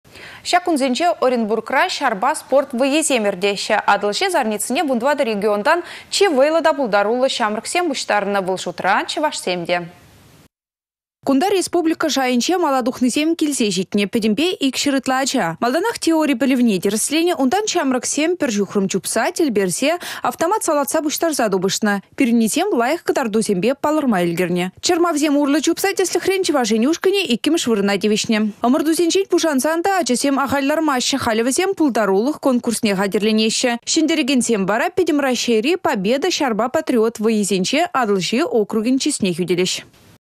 «Атăлçи зарници-2017»
Çак кунсенче Оренбургра çарпа спорт вăййисем иртеççĕ. «Атăлçи зарницине» 14 регионти чи вăйлă та пултаруллă çамрăксем пуçтарăннă. Вăл шутра чăвашсем те.